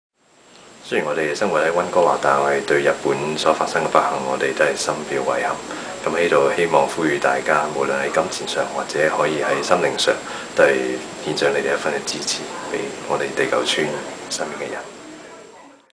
沈安麟 Raymond Sham 呼籲大眾為日本地震海嘯災民伸出援手
請立刻伸出援手，你的慷慨捐款可幫助日本災民解決燃眉之急及重建家園，捐款途徑可查詢網址